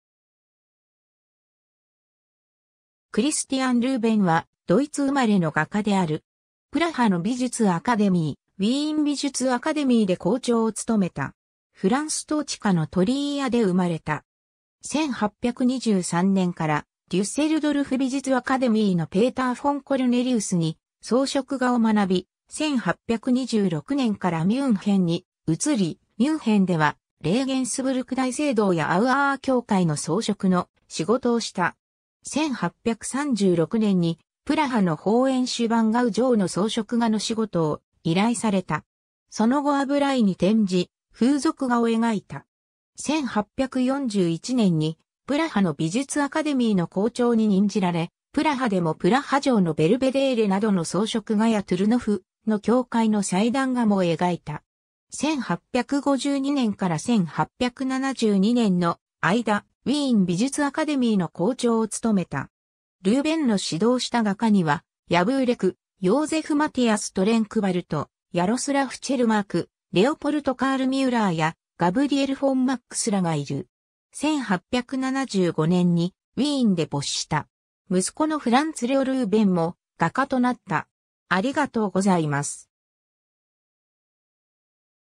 クリスティアン・ルーベン
[ クリスティアン・ルーベン（Christoph Christian Ruben、1805年11月30日 - 1875年7月9日）は、ドイツ生まれの画家である。
プラハの美術アカデミー、ウィーン美術アカデミーで校長を務めた。
 フランス統治下のトリーアで生まれた。
1823年からデュッセルドルフ美術アカデミーのペーター・フォン・コルネリウスに装飾画を学び、1826年からミュンヘンに移り、ミュンヘンではレーゲンスブルク大聖堂やアウアー教会 (Auer Kirche) の装飾の仕事をした。
 1836年にプラハのホーエンシュヴァンガウ城の装飾画の仕事を依頼された。
その後油絵に転じ、風俗画を描いた。
1841年にプラハの美術アカデミーの校長に任じられ、プラハでもプラハ城のベルヴェデーレ（夏の離宮）などの装飾画やトゥルノフの教会の祭壇画も描いた。
 1852年から1872年の間、ウィーン美術アカデミーの校長を務めた。
 ルーベンの指導した画家にはヤヴーレク（Karel Javůrek:1815年 - 1909）、ヨーゼフ・マティアス・トレンクヴァルト（1824年 - 1897年）、ヤロスラフ・チェルマーク（1830年 - 1878年）、レオポルト・カール・ミュラー（1834年 - 1892年）やガブリエル・フォン・マックス（1840年 - 1915年）らがいる。
 1875年にウィーンで没した。
息子のフランツ・レオ・ルーベン（Franz Leo Ruben:1842年 – 1920年）も画家となった。